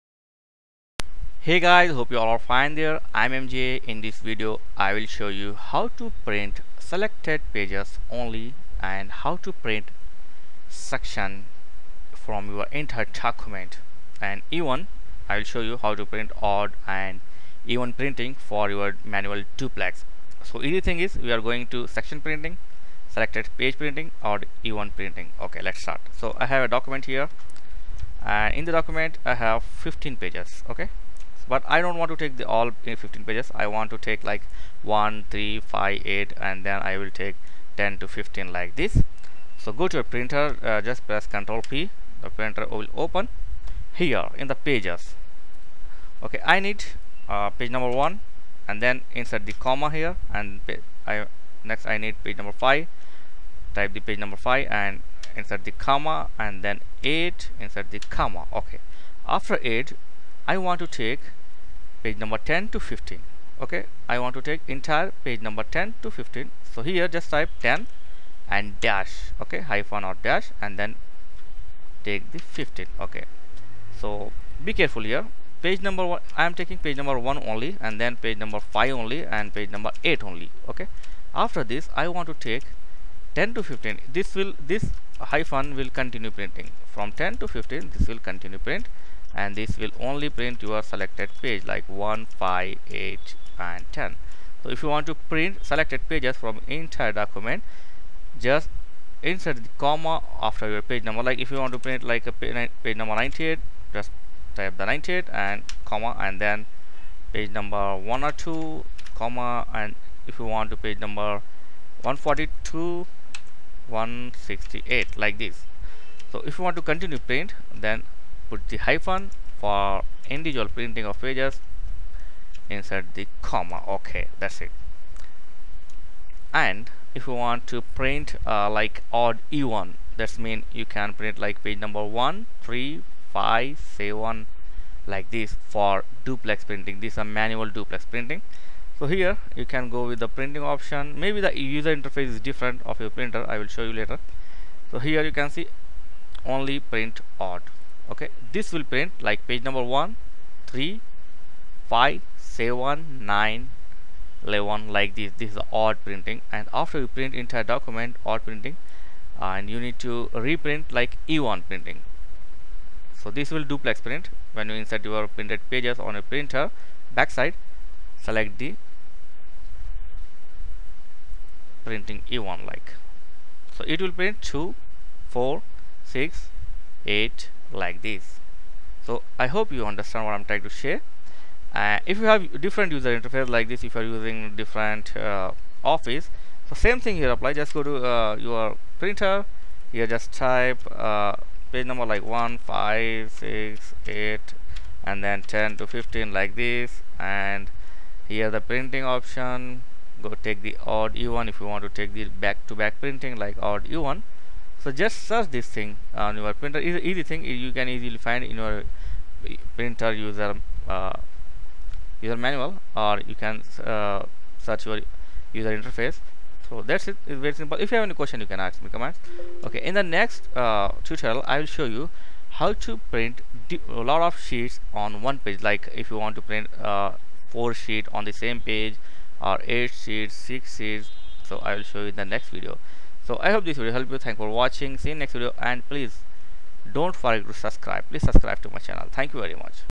How to Print Only Selected Pages, Section Print, Odd Even,
how to print select pages only, section printing and odd even print from enter document,